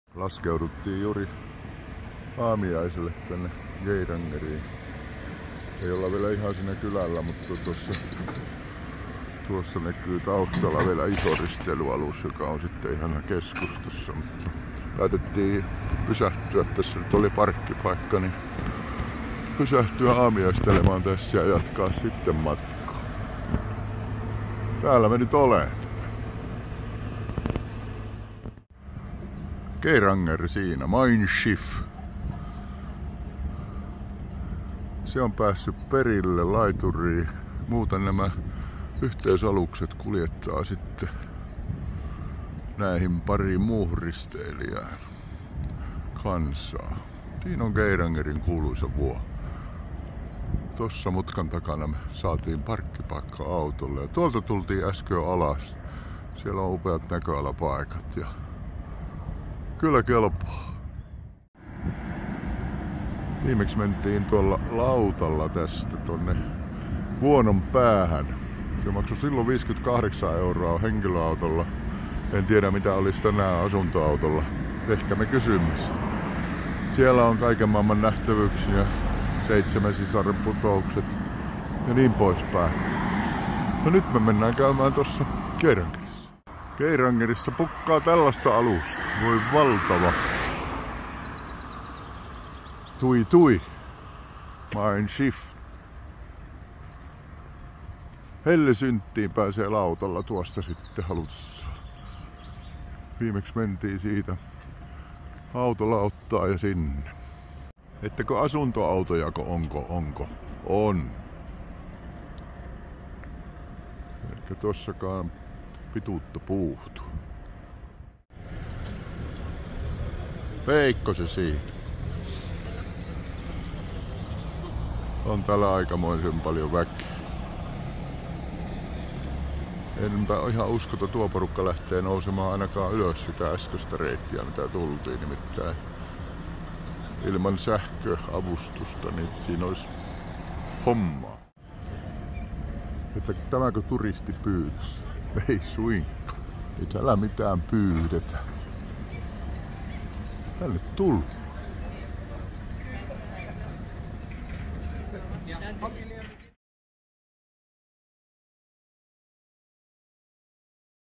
Geiranger Norja
National Geographic Traveler -lehti Geirangerinvuono maailman kauneimmaksi UNESCO:n maailmanperintöluettelon kohteeksi.
Lue lisää Geirangerin matkailukohteista blogissa